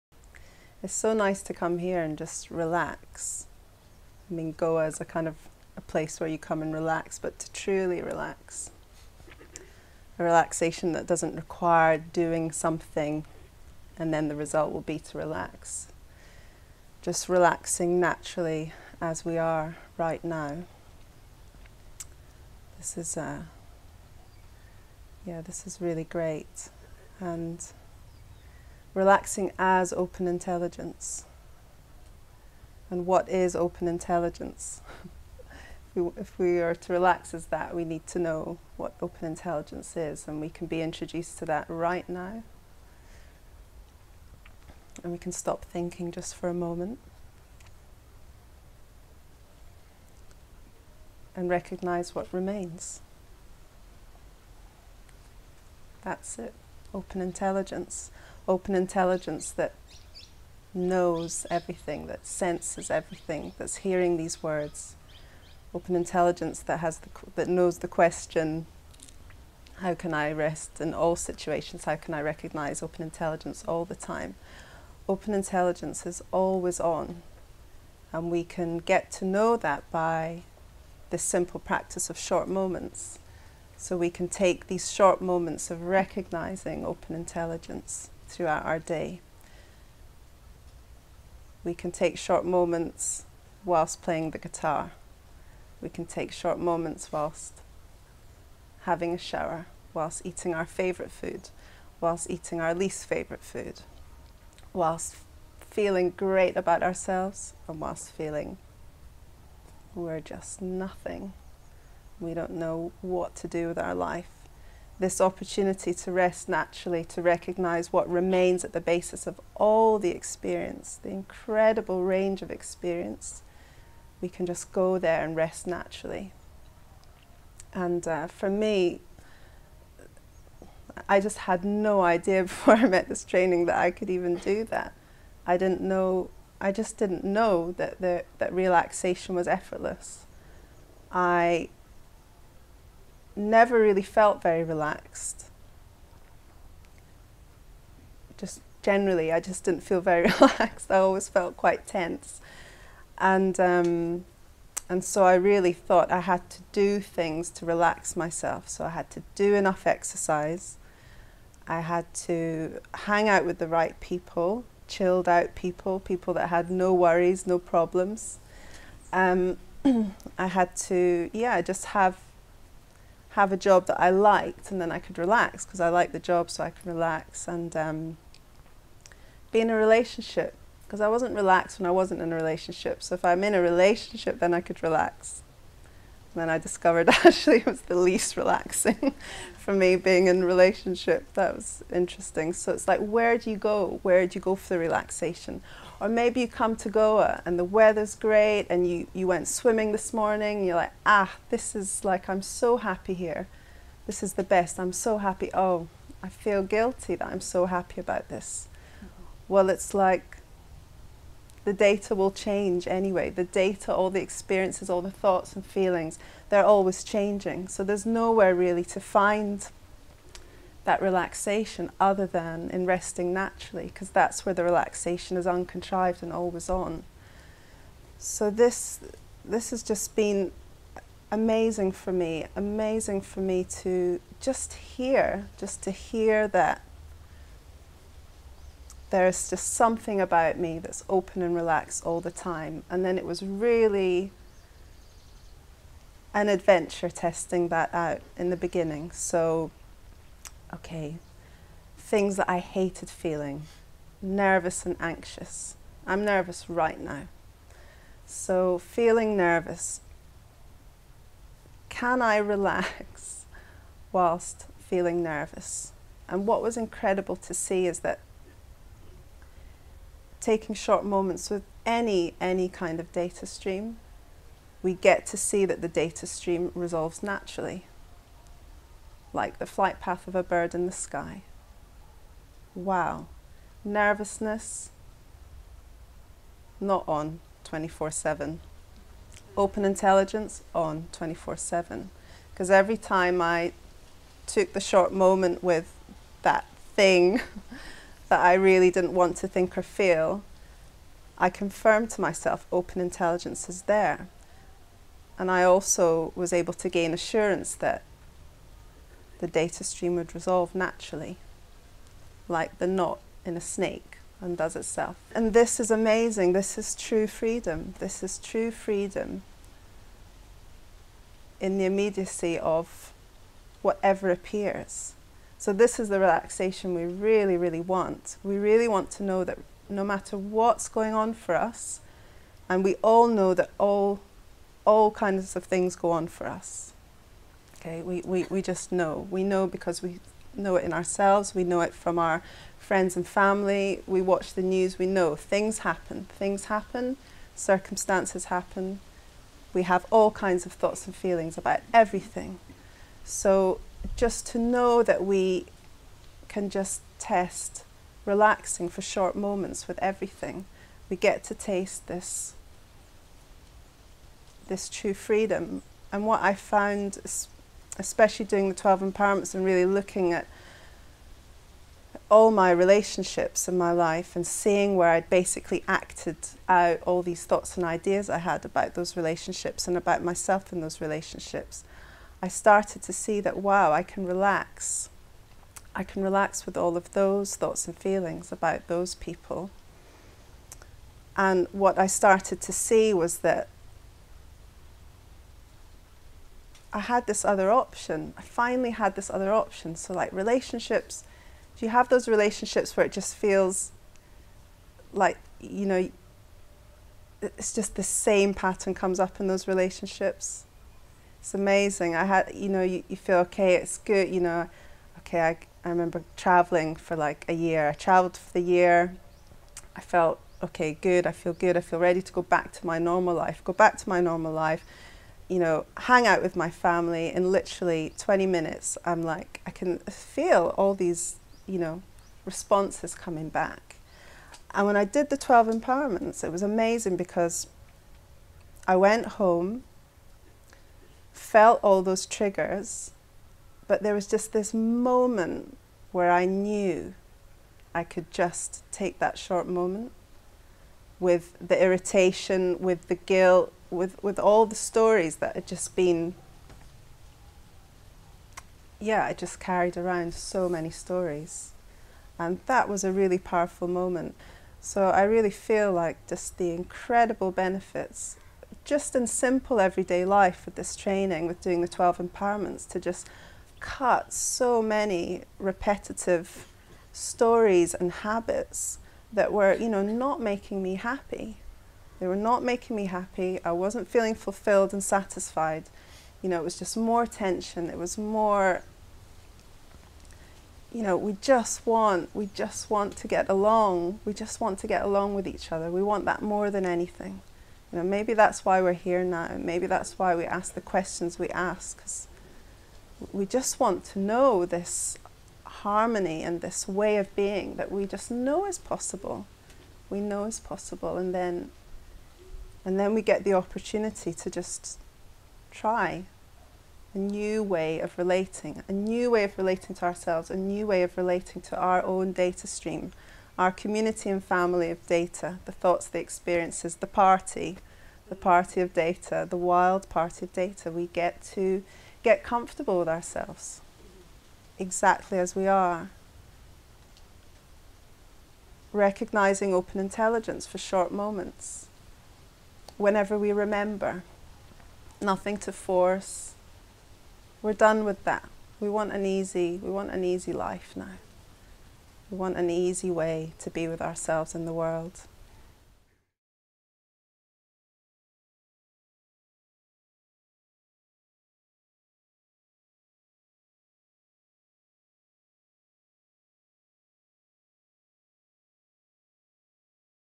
How to Relax in All Circumstances: Sonia Omar | Balanced View
Be introduced to how you can access consistent inner peace, wisdom and fulfilment in your life.

This talk was given at the Balanced View Center in Goa on Feb 7, 2018